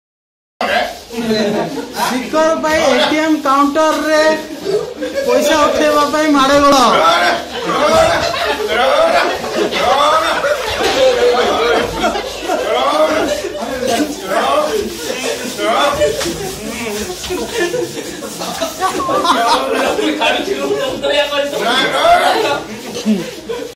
SBI ATM counter at Sikarpai for withdrawal new bank notes
How the people are reacting while withdrawal SBI ATM COUNTER SIKARPAI